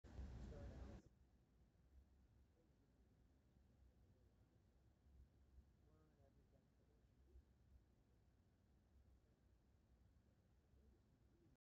riding through snowy Cascades on Amtrak
on the way from Chicago to Seattle